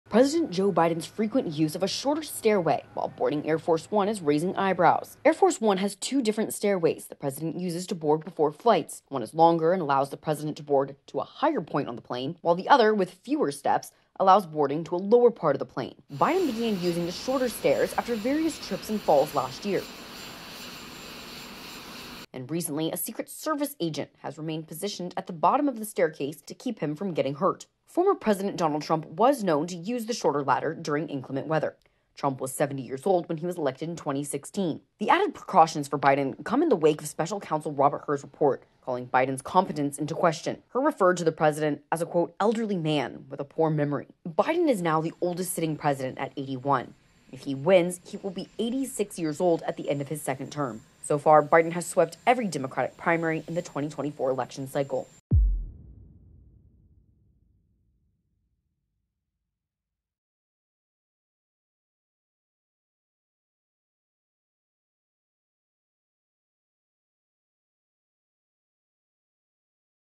Biden’s precautions boarding Air Force One raise questions
President Joe Biden’s frequent use of the shorter ladder while boarding Air Force One has raised eyebrows, and a Secret Service agent’s position at the base of the stairs has prompted new speculation.

Air Force One has two different ladders the president uses to board before flights. One is longer and allows the president to board to a higher point on the plane, while the other, with fewer steps, allows boarding to a shorter part of the plane. Biden began using the shorter stairs after various trips and falls last year. And recently, a Secret Service agent has remained posted at the bottom of the staircase, the New York Times reported, ostensibly to keep him from getting hurt.